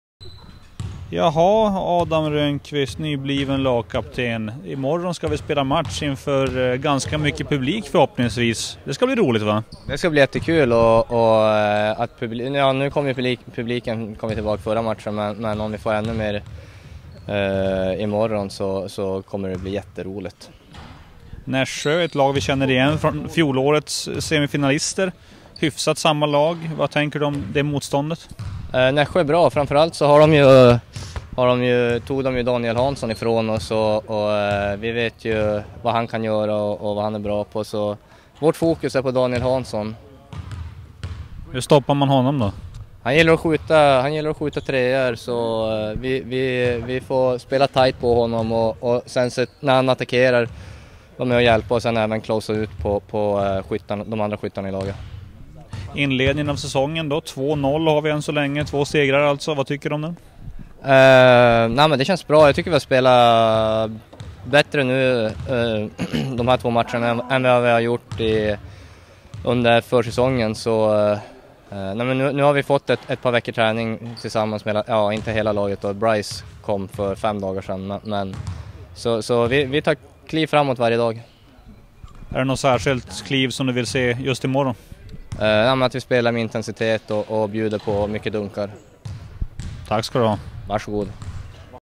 Lagkapten Adam Rönnqvist inför första matchen utan publikrestriktioner
Intervju med BC Luleås lagkapten Adam Rönnqvist inför första matchen utan publikrestriktioner.

Matchen spelas fredagen 1 oktober mot Nässjö Basket.